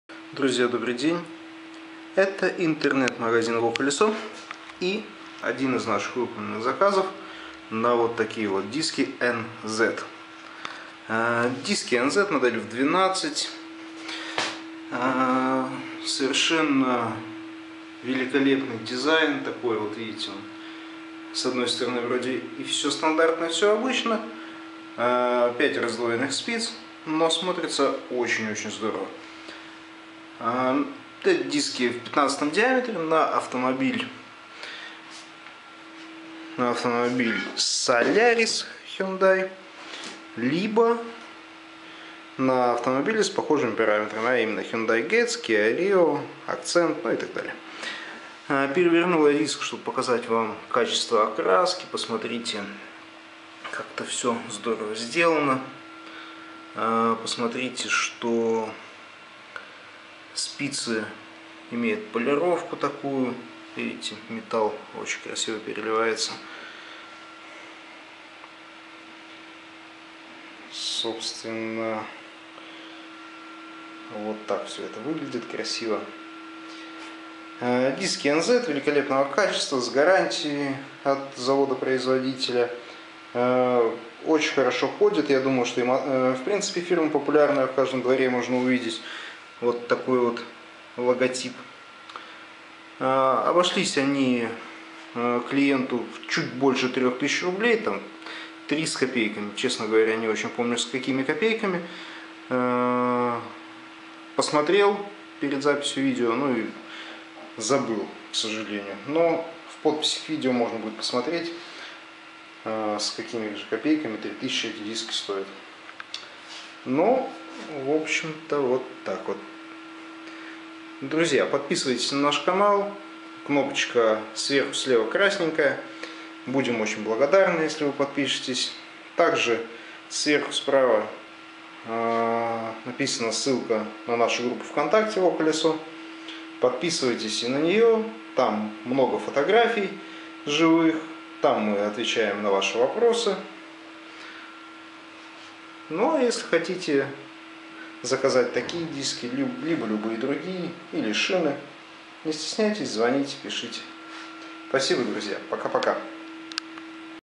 Диски NZ Hyundai Accent, Solaris, Getz, Kia Rio и др. /// обзор
Литые диски NZ R15 на Hyundai Accent, Solaris, Getz, Kia Rio и др.

Интернет-магазин "ВоКолесо". Шины/Диски
Доставка по всей России транспортными компаниями
Офис: г. Ростов-на-Дону, ул. Петренко, д.4